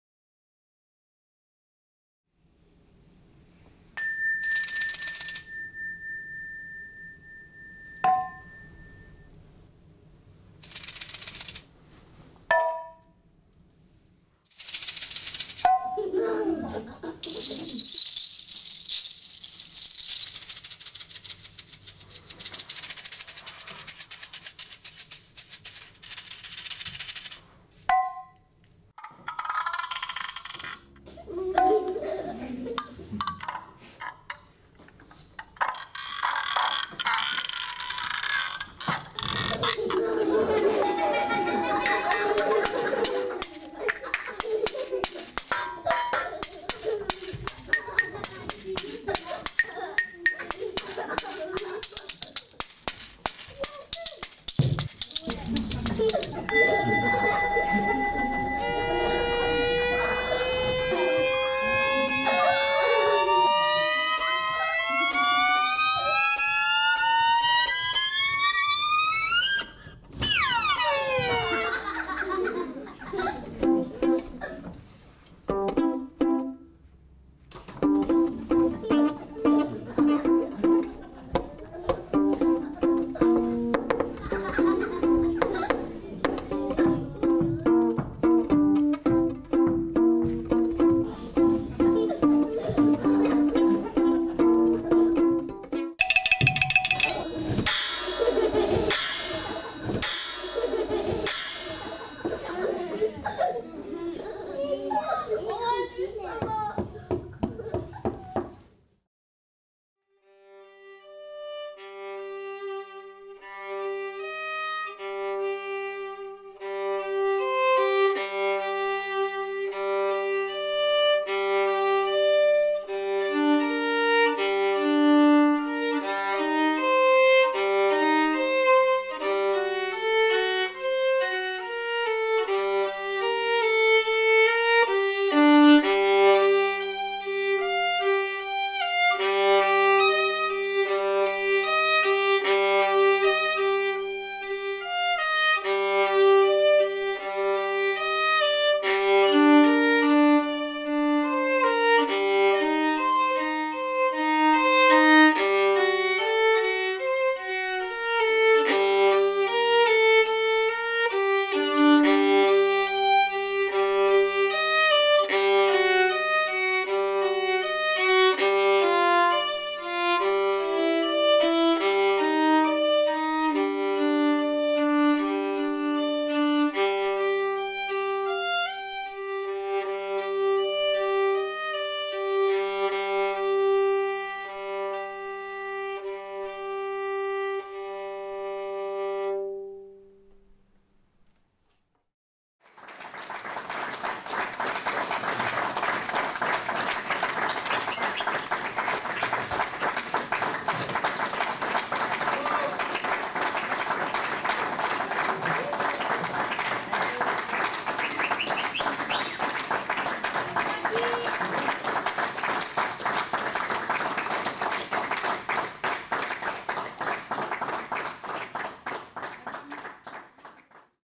TOYO
CIRQUE & MUSIQUE - Tout public dès 3 ans
MERCREDI 31 MARS 2021 à 10h

+ séances scolaires les JEUDI 1er & VENDREDI 2 AVRIL 2021 à 9h30 & 10h30
THEÂTRE MADELEINE-RENAUD TAVERNY